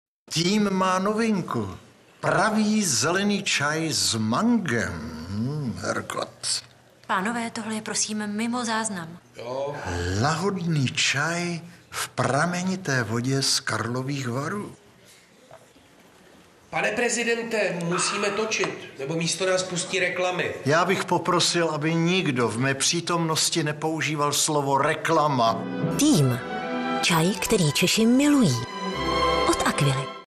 Aquila Team: Prezident Karel Gott & mango [reklama]
- Karel Gott jako prezident České republiky v reklamě na čaj Aquila Team, novinka mango. Čaj, který Češi milují.